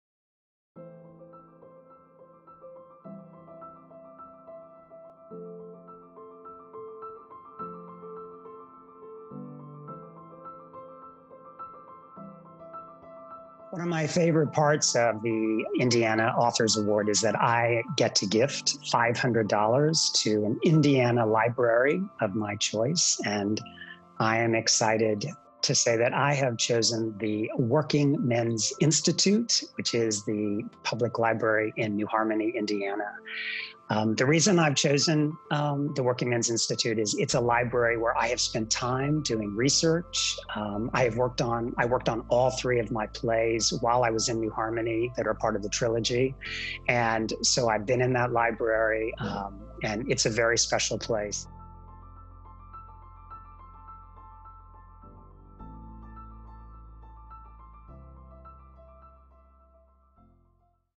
James Still selects Working Men’s Institute
Winners of the Eugene and Marilyn Glick Indiana Authors Awards have chosen five Indiana libraries to receive $500 donations in their honor.

In addition to winning a $5,000 cash prize and a one-of-a-kind hand-crafted limestone award, the winning authors were each given the opportunity to make a $500 donation to an Indiana library of their choice.

IndianaAuthorsAwards.org